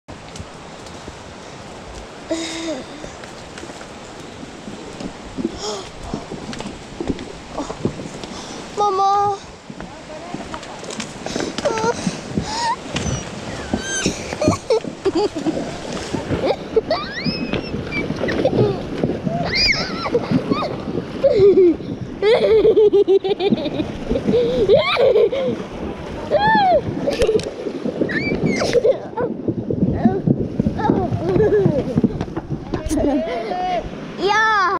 A cold, thrilling experience with Icelandic horses
Crossing a small river with icy-cold water is an easy task for these amazing horses. The experience is a great pleasure for one of the riders, an eight year old that is tougher than lots of grownups :-)